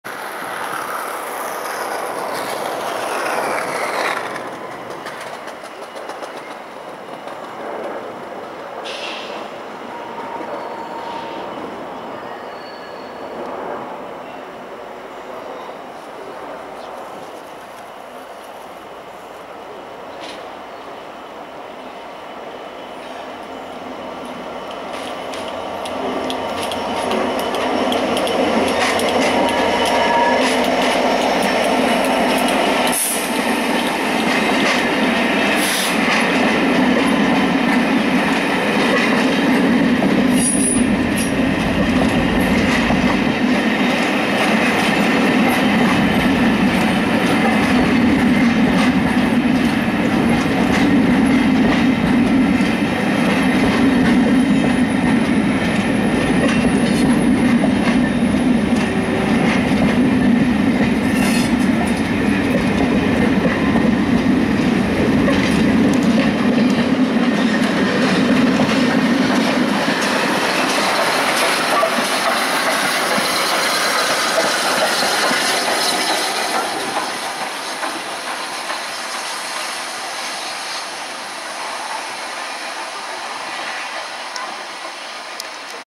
67024 bringing the VSOE with 35028 Clanline back from London Victoria 22/03/2014
67024 bringing the VSOE with 35028 Clanline back from London Victoria passing Stewarts Lane Junction straight onto a turning move 22/03/2014